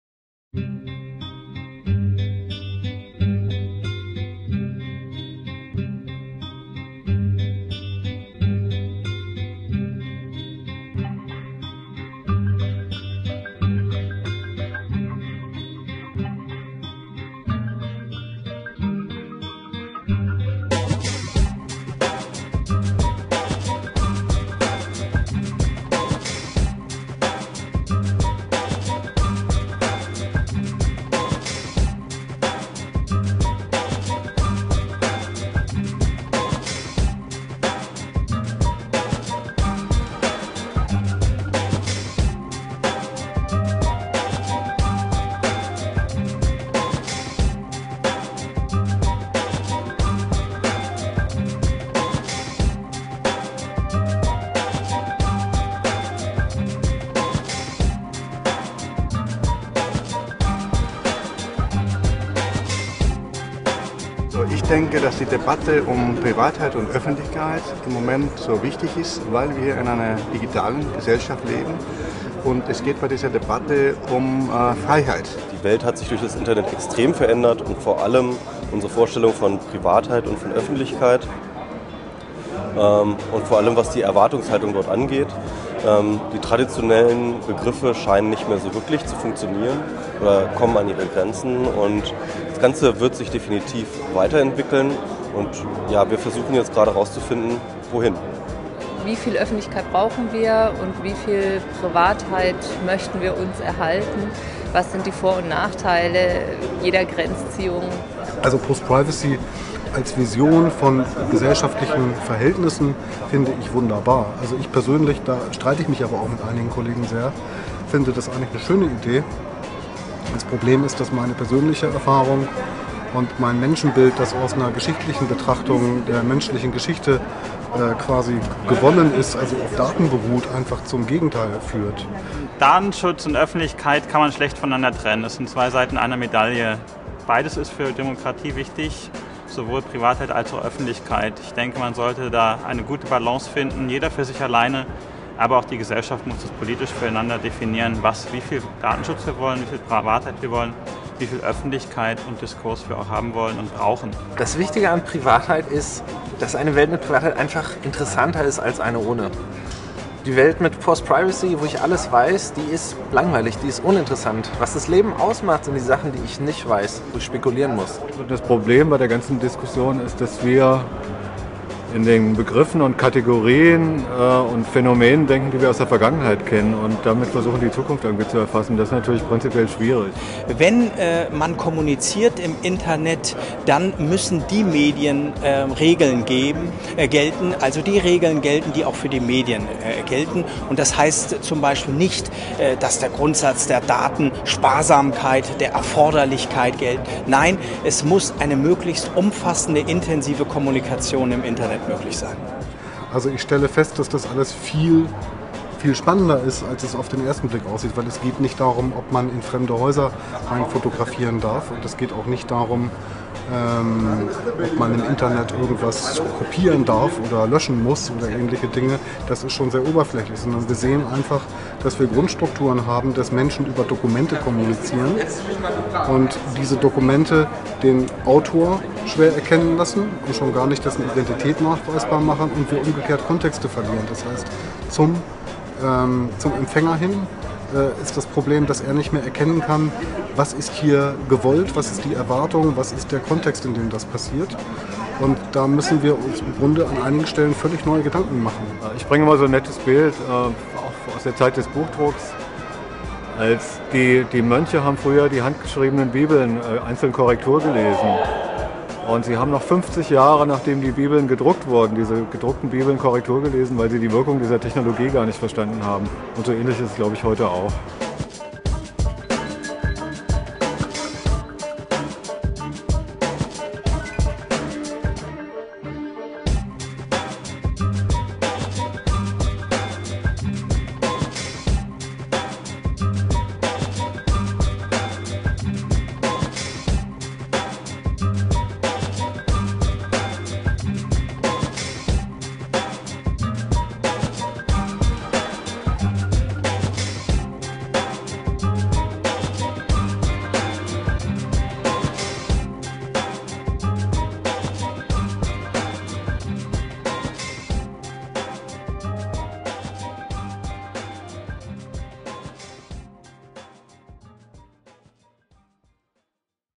Co:llaboratory Impressionen vom Abschlussworkshop "Privatheit und Öffenlichkeit"
Am 16. September 2011 stellte der Expertenkreis des Internet & Geselschaft Co:llaboratory die vorläufigen Ergebnisse zur Initiative "Privatheit und Öffentlichkeit" in der Landesvertretung Sachsen-Anhalt in Berlin vor. In diesem Video sprechen einige Teilnehmer die vierten Initiative zu ihrem Verständnis von Privatheit und Öffentlichkeit.